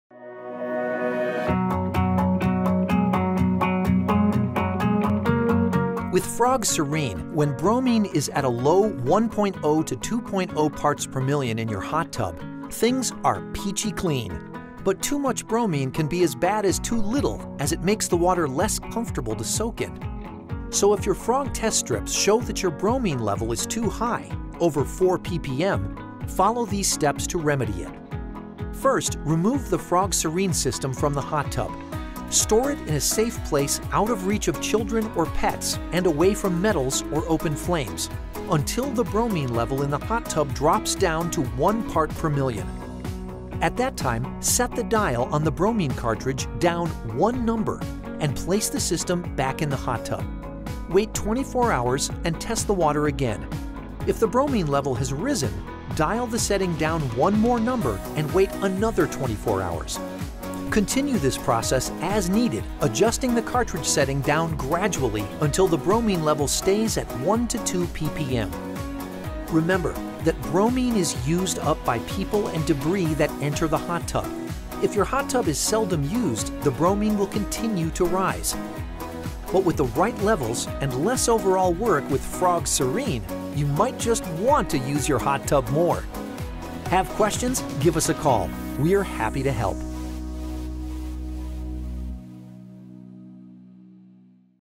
How to Lower Bromine Levels in a Hot Tub with FROG Serene
1. Remove the Bromine Cartridge from the spa.
If the bromine level is over 4.0 ppm, remove the Bromine Cartridge and store in a safe place away from children, pets, metals or open flame until the level drops to 1.0 ppm.

2. Determine if the bromine cartridge setting is too high. When the bromine level has come down to 1.0 ppm, replace the cartridge in the spa at one setting lower than it was before. Test the water in 24 hours to determine if it has risen, declined or remained the same. Adjust the cartridge accordingly until 1.0 to 2.0 ppm is maintained.

3. Evaluate your spa usage. If the spa is not being used very often, then the need for bromine (bromine demand) is lessened. You may need to remove the system from the spa periodically to avoid continuous over bromination.